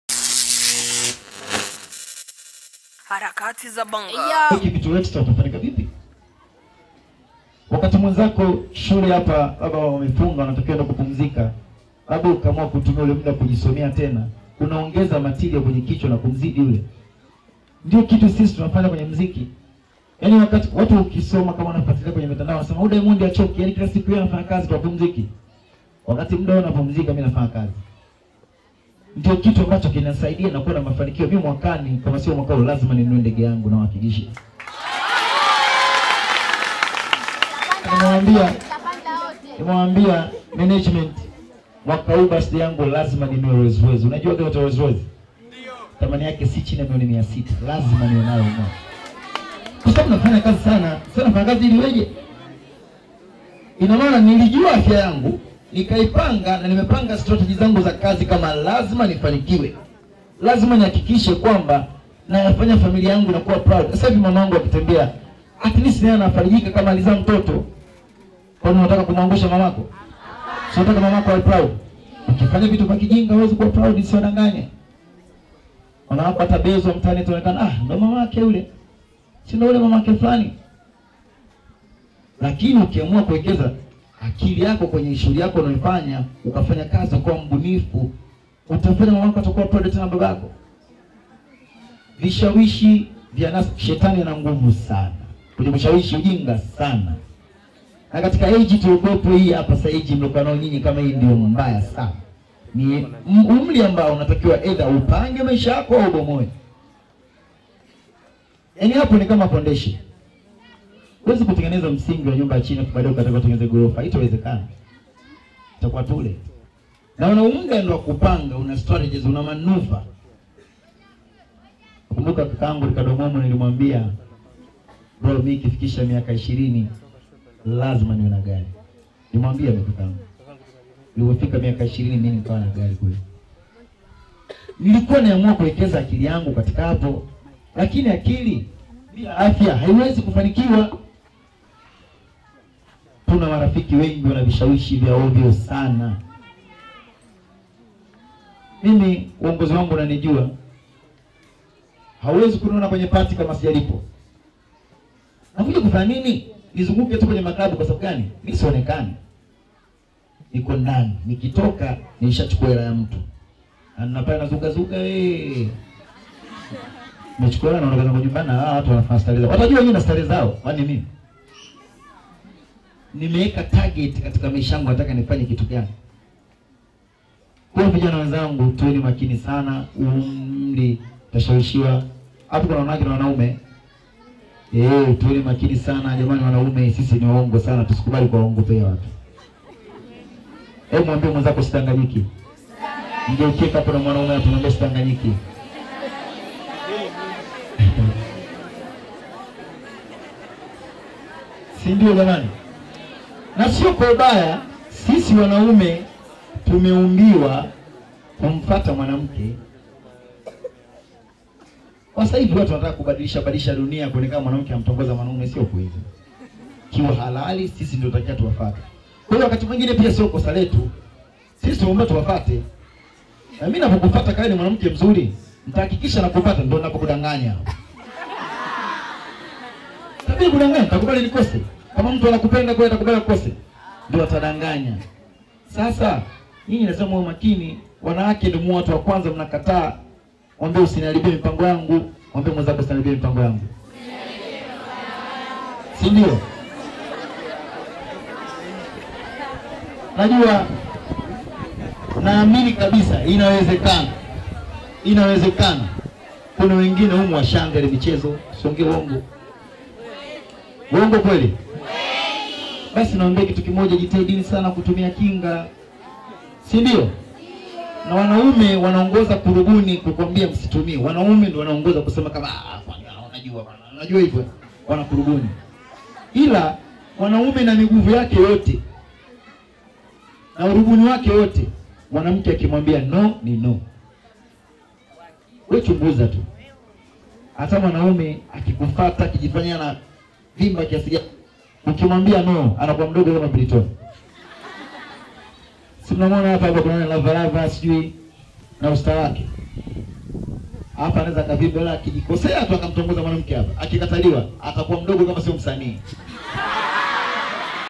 DIAMOND atangaza kununua ndege yake mwaka huu/Aeleza sababu ya kutokuwa na marafiki
Mwanamziki Naseeb Abdul maarufu kama Diamond Platnumz siku ya leo alitembrlra shule ya Secondary Kiwanja cha Ndege na kuongea na wanafunzi wa shule hiyo kuhusiana na maambukizi ya virusi vya UKIMWI.